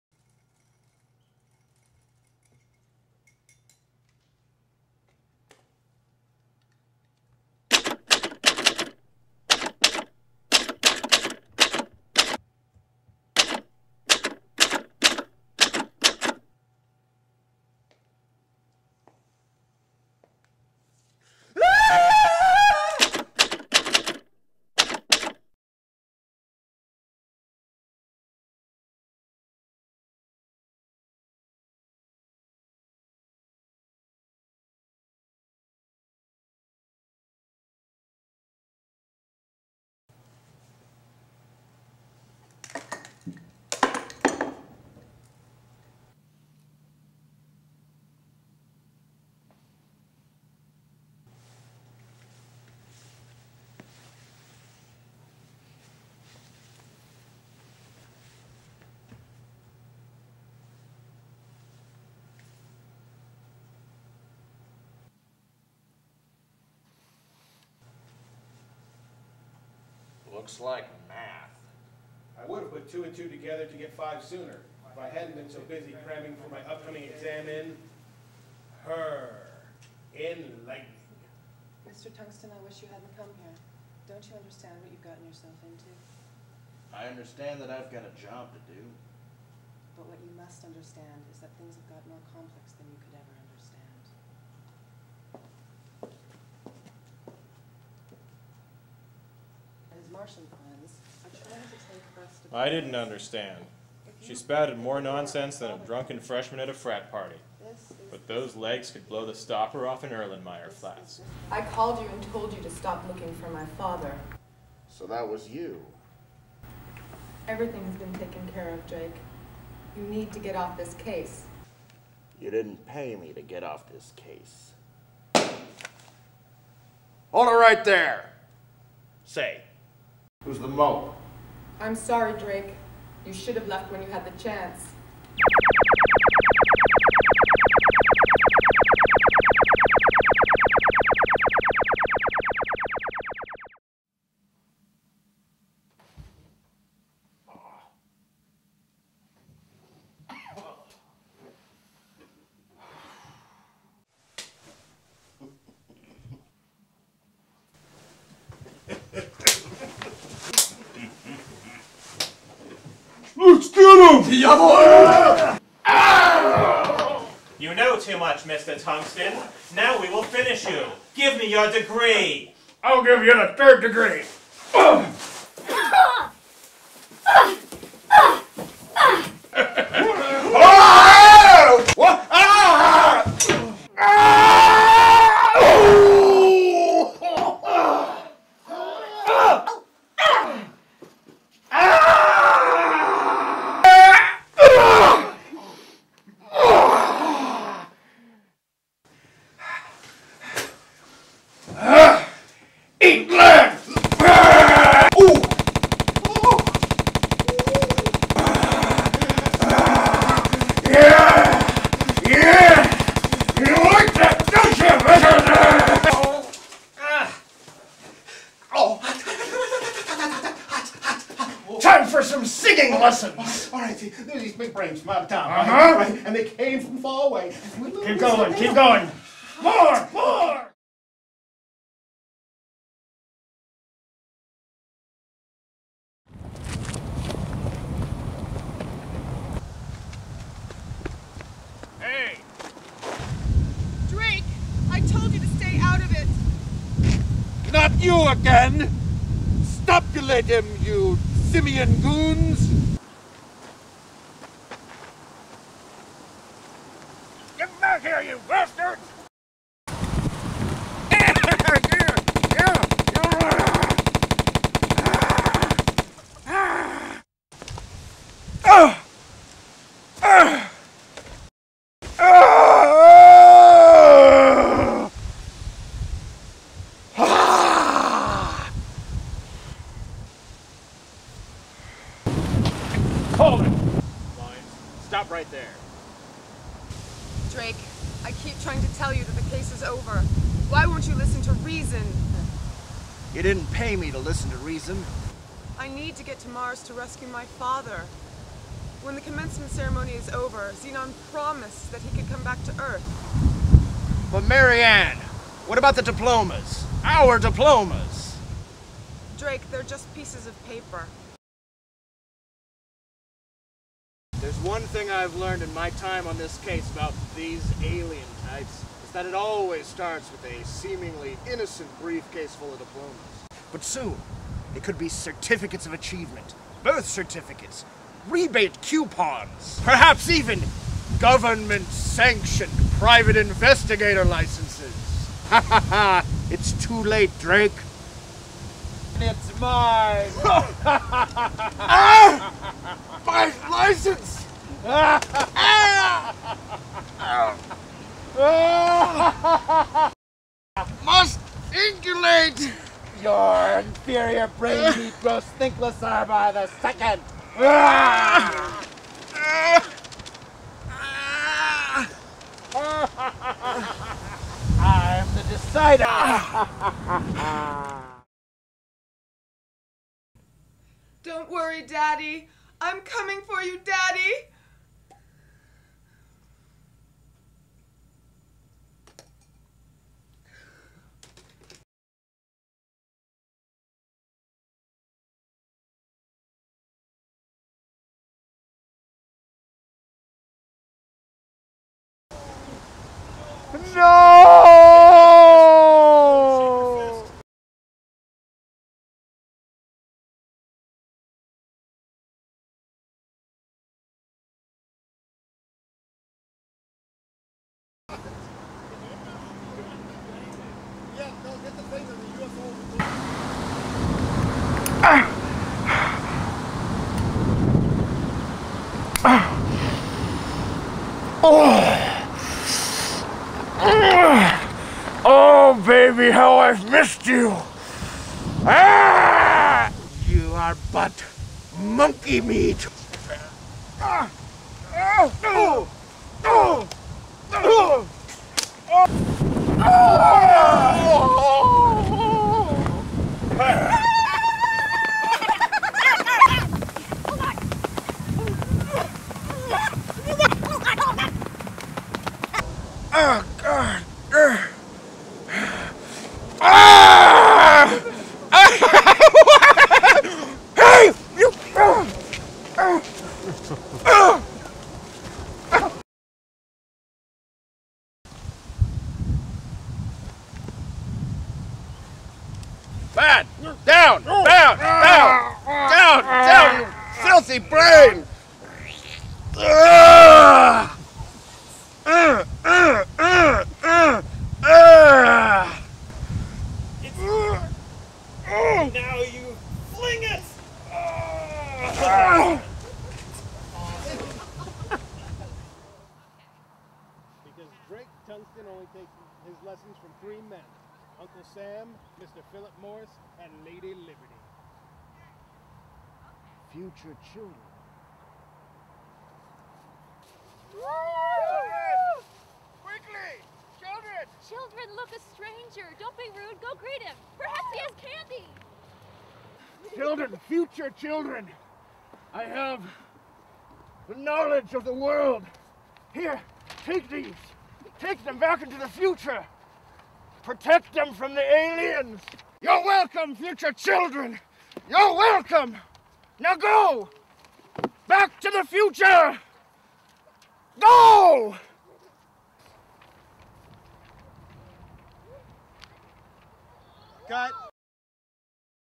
Drake Tungsten in Brain Drain (Rough Assembly)
This is the ROUGH ASSEMBLY of the SECOND Drake Tungsten short.

Boy is it odd without the voiceover narration.  Hopefully Jason Topolski (Drake) and I will record that sometime and I'll cut it in...

Drake Tungsten (name ripped from the same MST3K episode that the singer ripped it from) was a comic noir created initially as part of a 48 hour film festival.

It starts out strong technically, and completely jumps the shark during the exterior shots when we lost our location, fought wind in the new one, tried a stupid filter trick that failed, and ran out of time and couldn't shoot 20% of the script.

But it's funny, and the potential for the character and world is still obvious even here.

I'd love to bring the character back someday, but until then...

This is the only partially-completed Drake short.